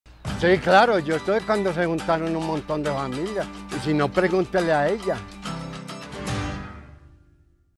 Bancolombia | Orígenes: Varias familias se juntaron para crear algo incríble, conócelo aquí.
¿Quieres saber más de esta historia de éxito?